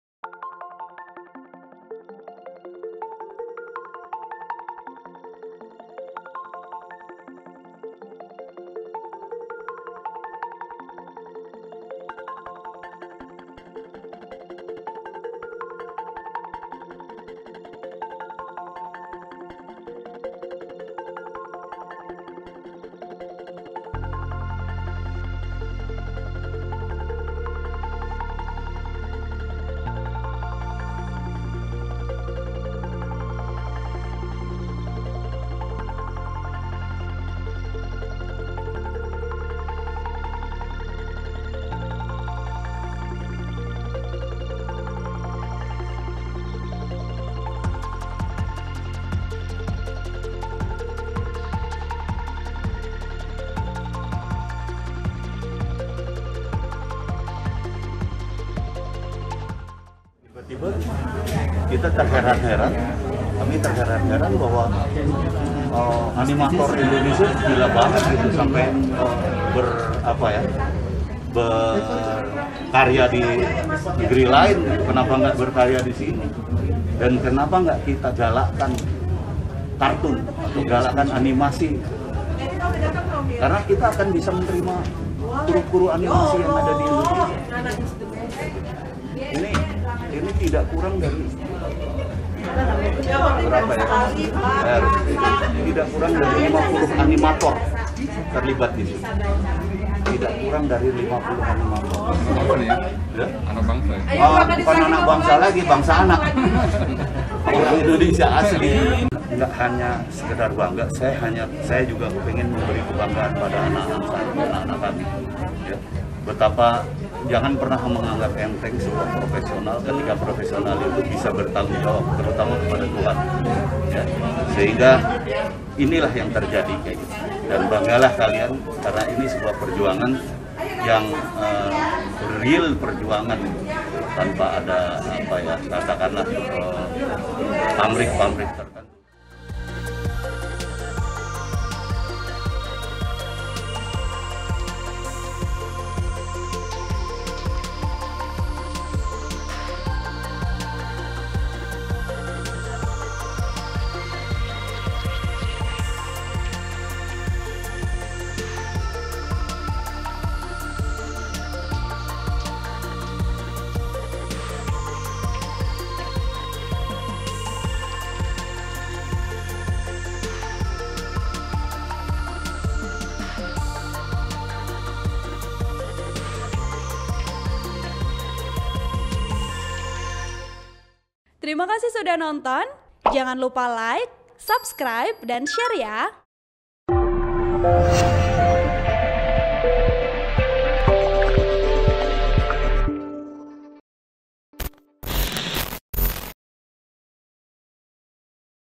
Warkop DKI Akan Tayang Dalam Versi Kartun, Begini Kata Indro Warkop
TRIBUN-VIDEO.COM - Warkop DKI akan tayang di layar lebar dalam versi kartun pada Desember 2022 mendatang.

Hal tersebut terungkap dalam Falcon Showcase 2022 di bioskop, Jalan H.R. Rasuna Said, Jakarta Selatan, Kamis (14/4/2022).

Menanggapi hal tersebut Indro Warkop mengatakan, proses pembuatan film itu melibatkan tidak kurang dari 50 animator asli Indonesia.

Indro Warkop mengaku, baru mengetahui banyak animator asli Indonesia yang berhasil mengeluarkan karya di negeri orang.

Hal tersebut menjadikannya sangat menyesal, oleh sebab itu ia berharap Warkop DKI Kartun bisa menjadi pemicu menggeliatnya film-film animasi Indonesia.

Sehingga animator-animator dalam negeri tidak perlu jauh-jauh mengekspresikan diri di negeri orang.

Dalam film Warkop DKI Kartun tersebut, yang menjadi pengisi suara di antaranya: Aliando Syarief, Randy Danistha, dan Adipati Dolken.

Editor Video : Ghozi Luthfi R